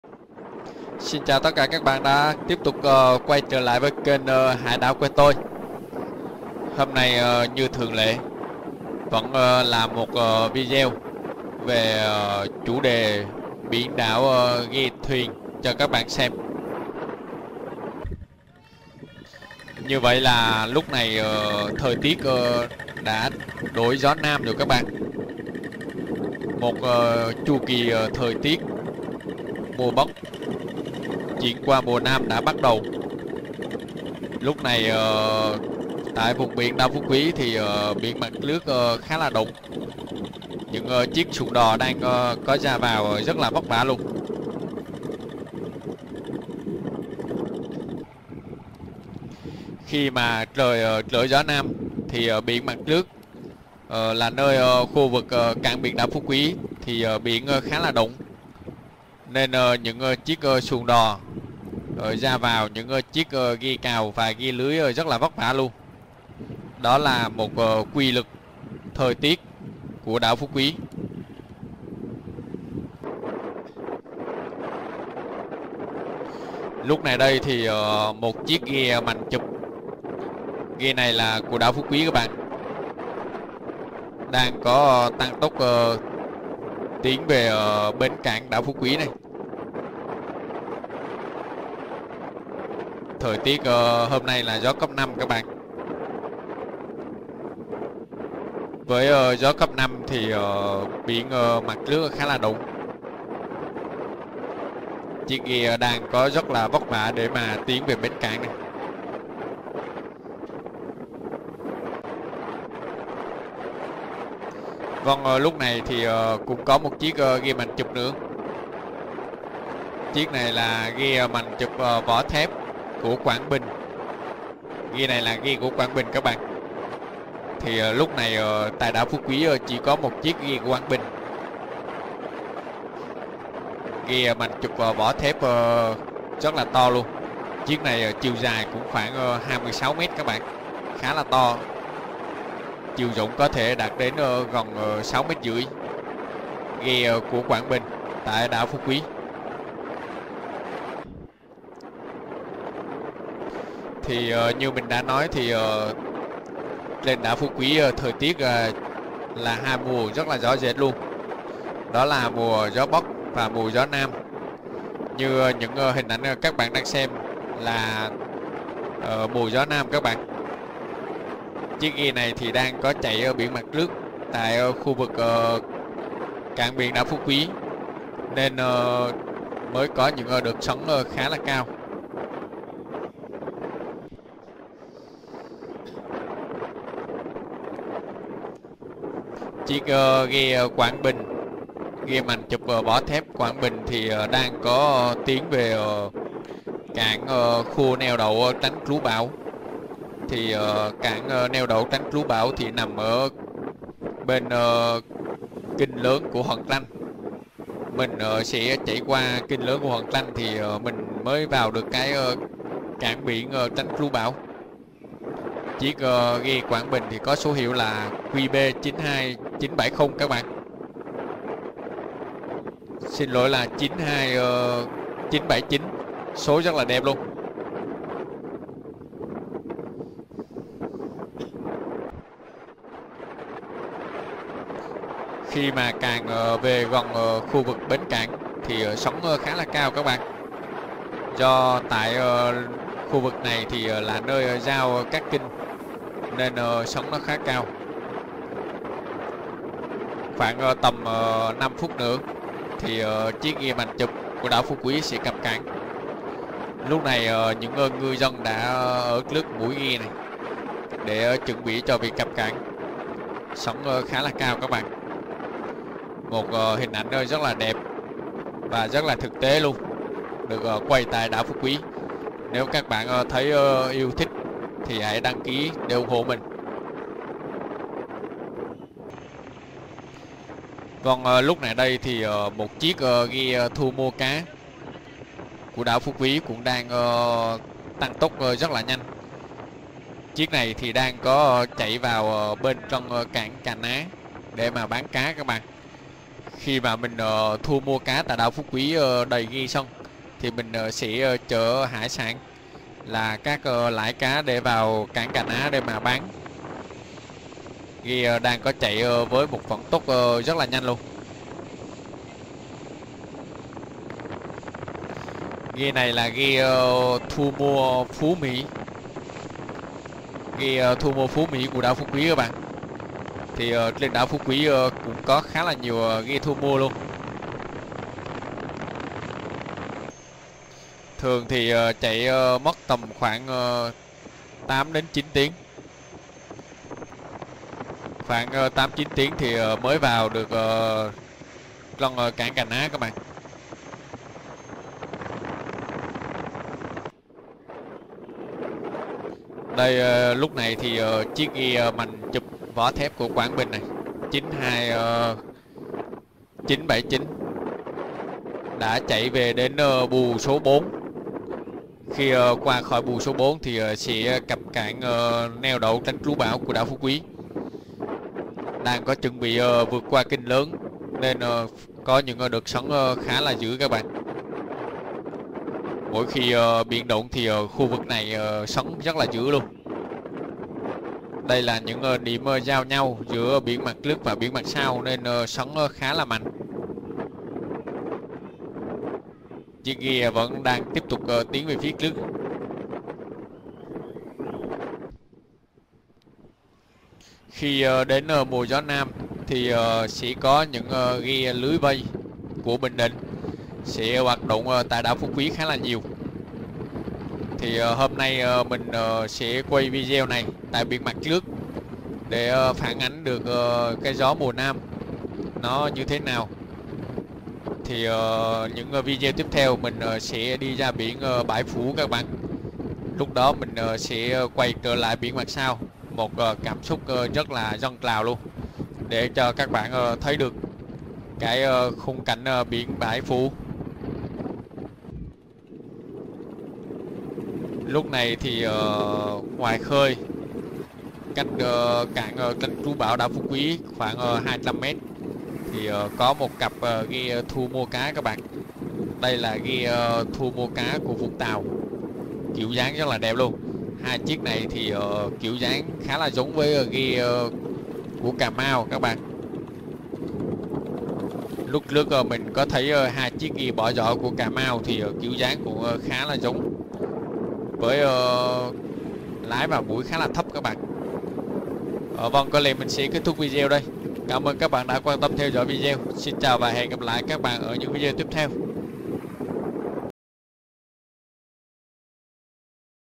Ghe mành chụp ngược gió nam
Ghe mành chụp đảo Phú Quý ngược gió nam về cảng.